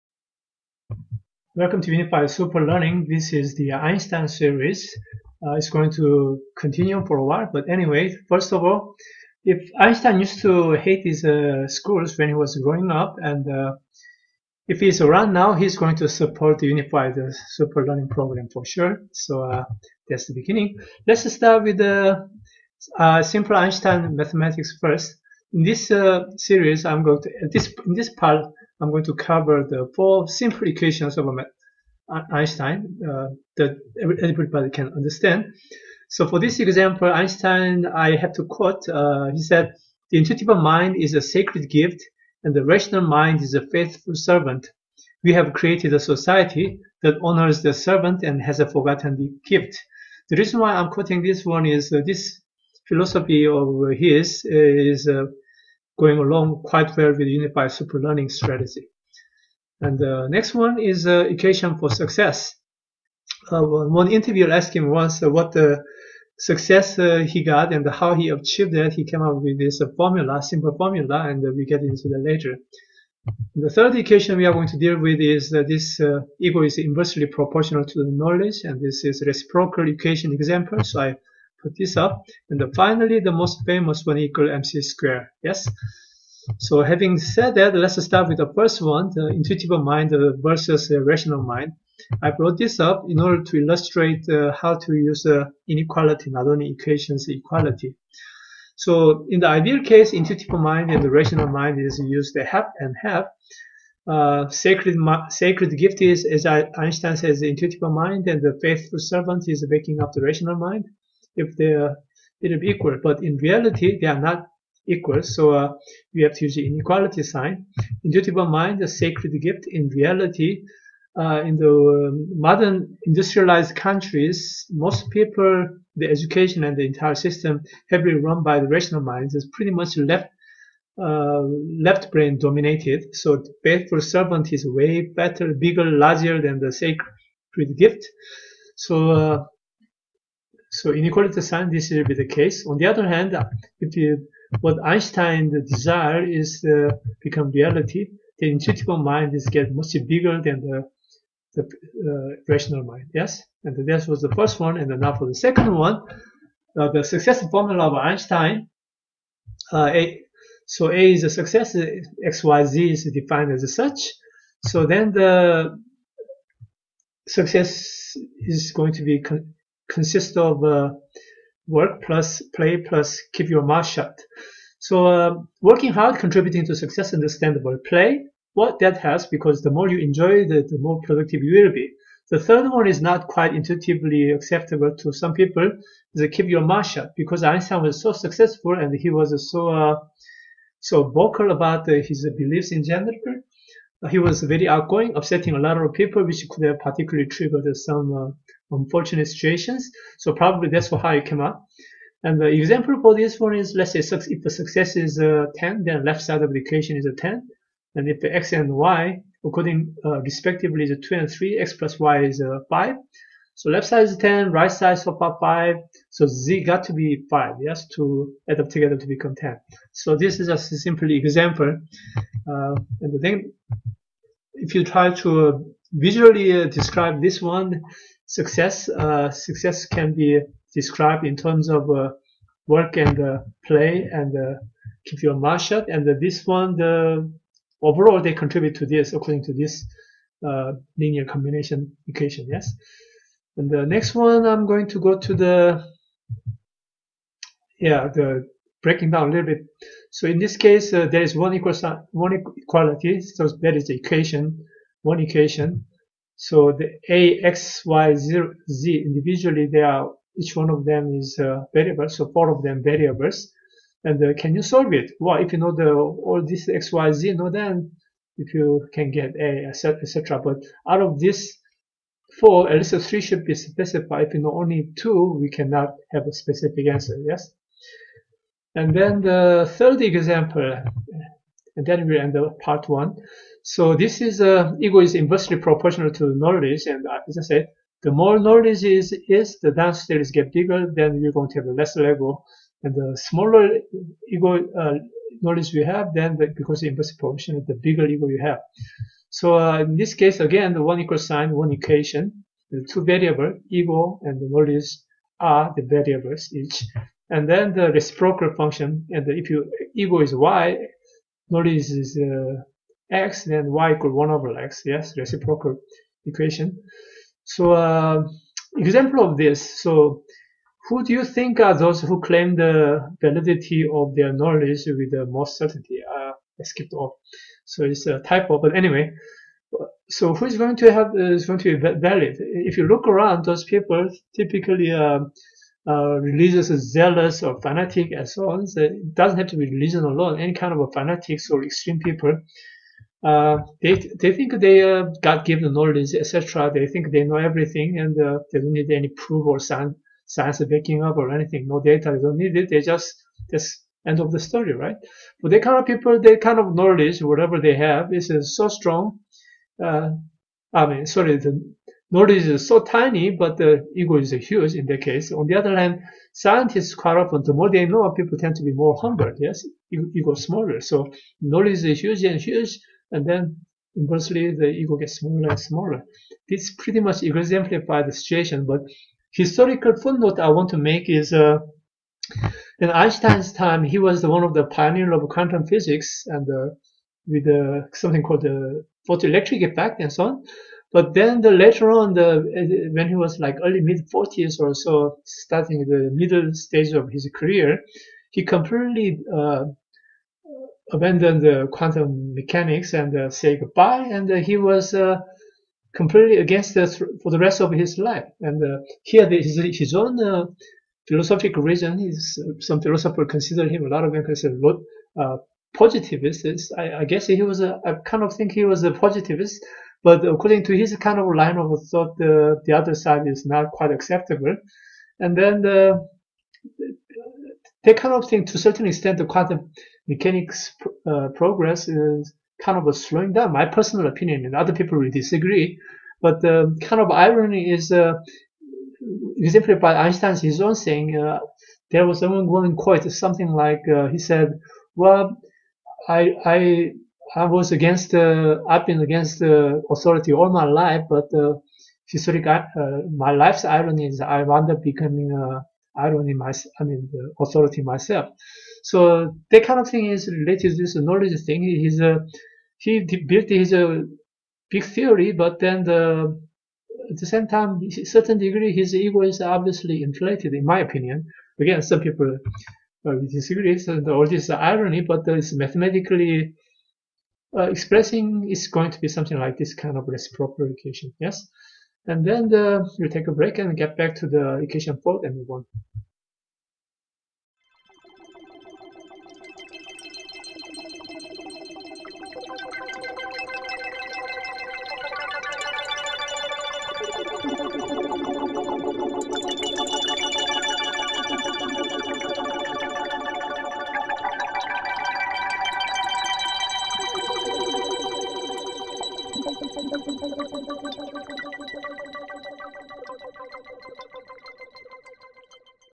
USL: Einstein quotes for fun math. Einstein for fun easy math series - Part1
You can learn more math with the anecdotal foot notes about Einstein.